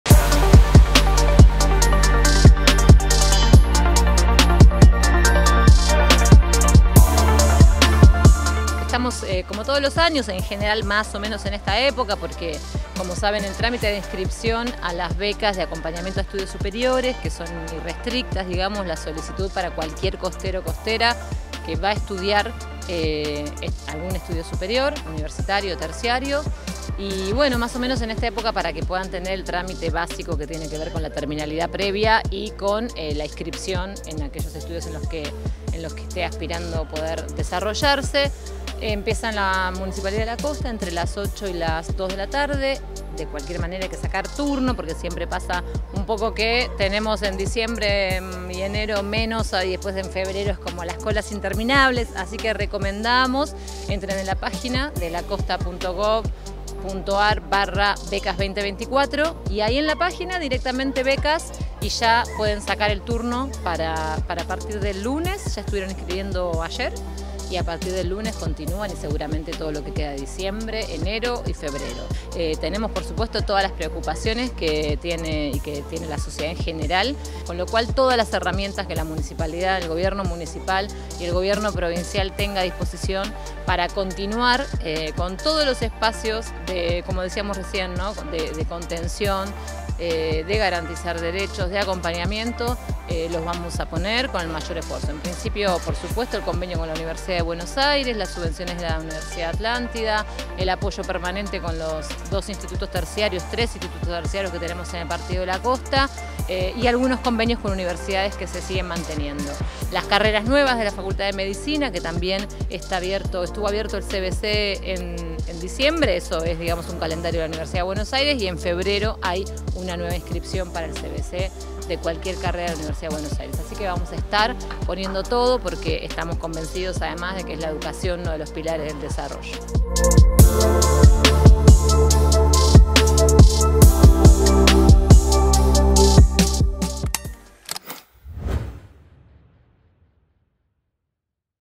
Abrió la inscripción para el programa Tramitá tu Beca
Abrió la inscripción del programa que permite renovar o solicitar el acompañamiento económico para quienes estudian una carrera de nivel superior. Para realizar el trámite de inscripción deben ingresar a lacosta.gob.ar/becas2024 y completar el formulario.